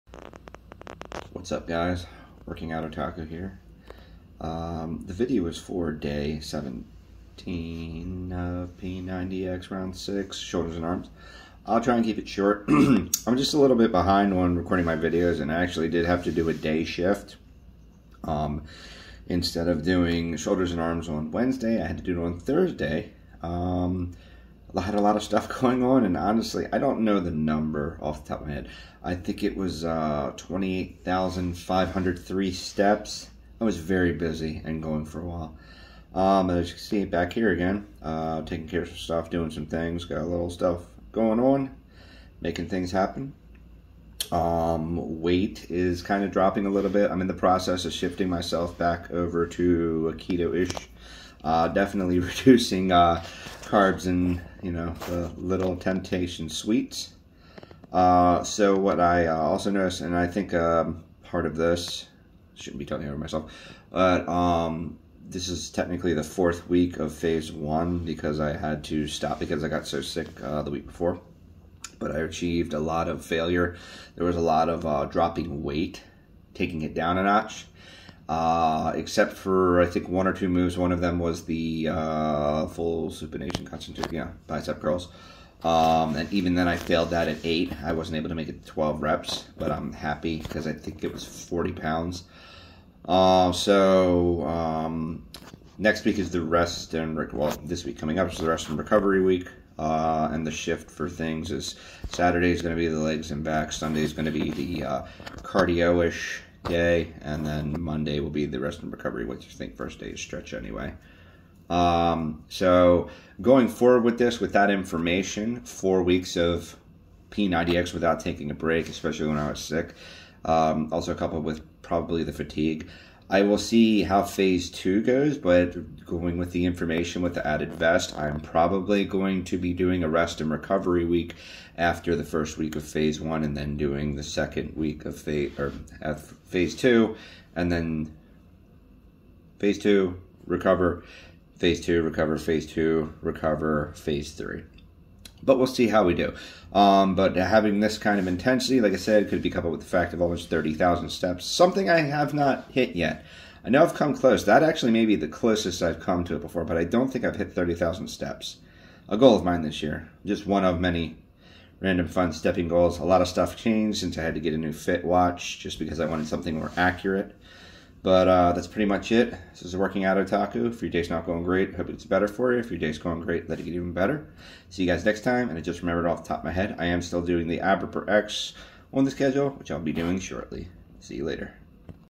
P90X round 6 day 17 fatiguing thru Shoulders and Arms
a struggle and fatiguing indeed. This is the first time I added a week to a phase of p90x. was it that or the 28k+ steps I did the day before? and perhaps the few hrs of sleep I got lol. better preparations coming for the next phase of p90x.